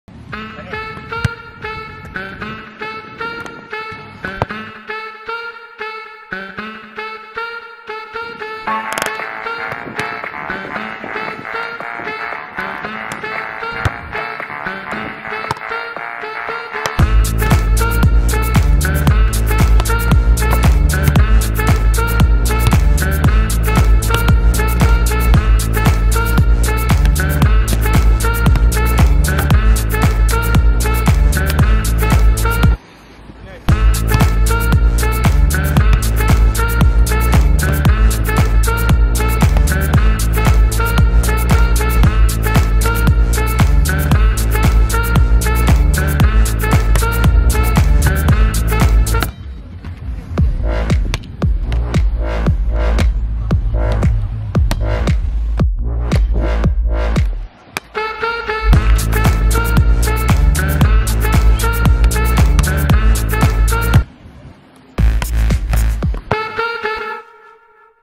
Cinematic Beach Volleyball. Canon m50 with kit lens 15-45mm
Highlights cut of beach valleyball